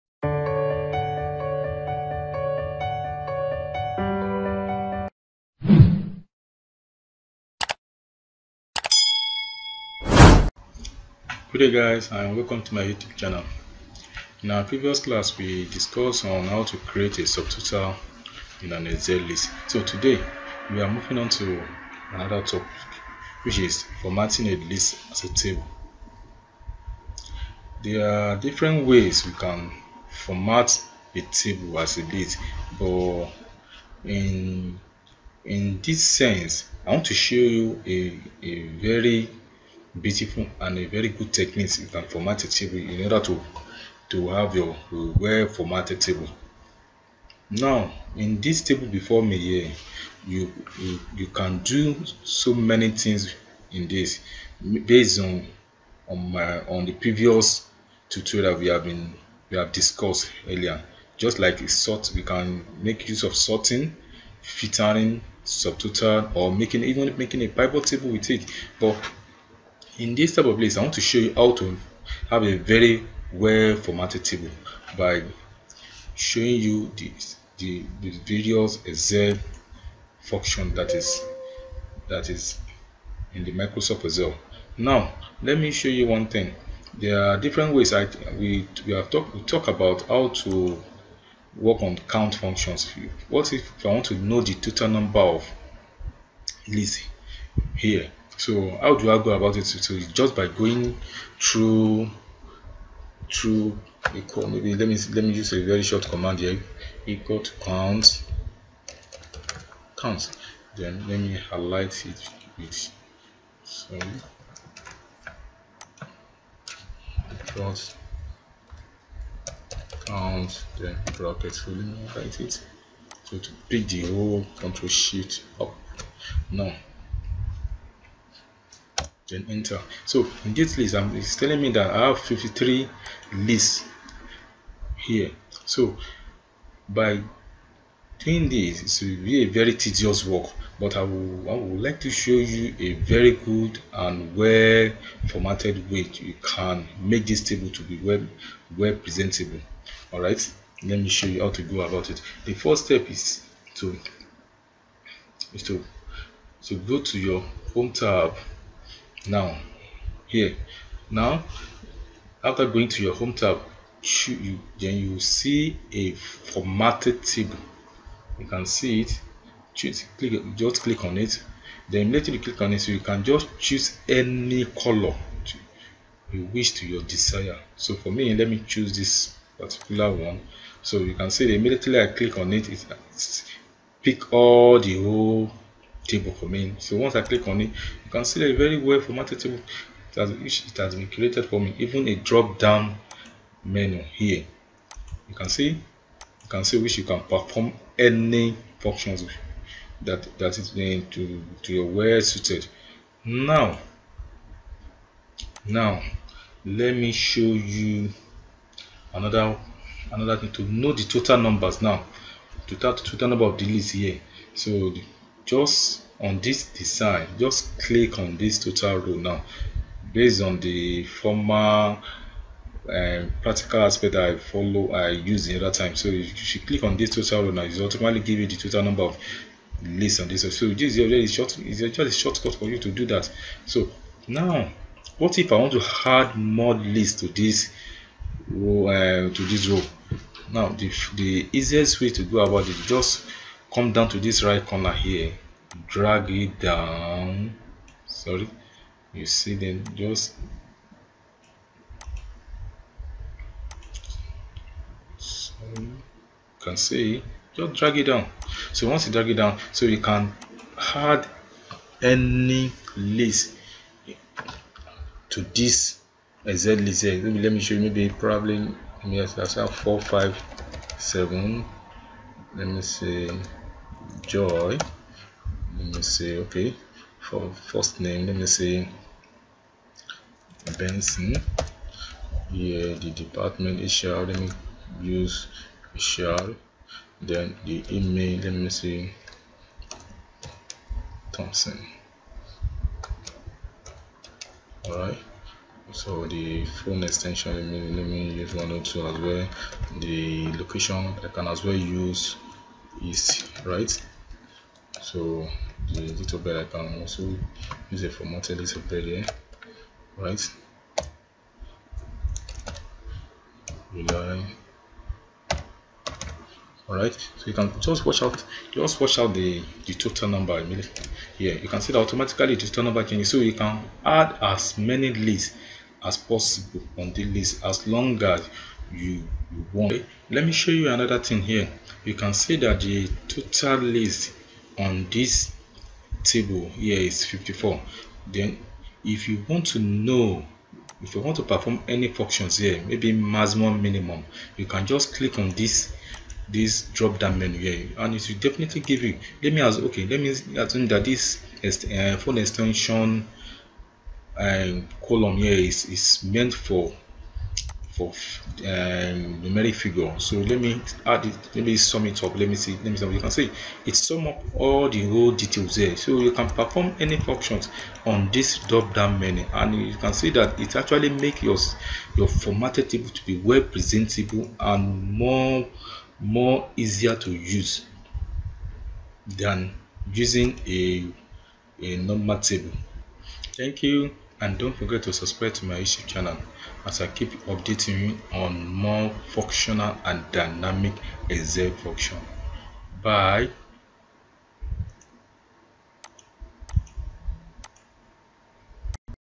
How To Format A List In An Excel As Table
Learn: The Quick & easy way on How To Format A List In An Excel As Table

In this video, you will learn:
1. How do I turn a table into a list in Excel
2. How do I format data in an Excel table

Just like regular formatting, tables can help to organize your content and make it easier for you to locate the information you need. To use tables effectively, you'll need to know how to format information as a table, modify tables, and apply table styles.

Once you have entered information into a spreadsheet, you may want to format it. Formatting your spreadsheet can not only improve the look and feel of your spreadsheet, but it also can make it easier to use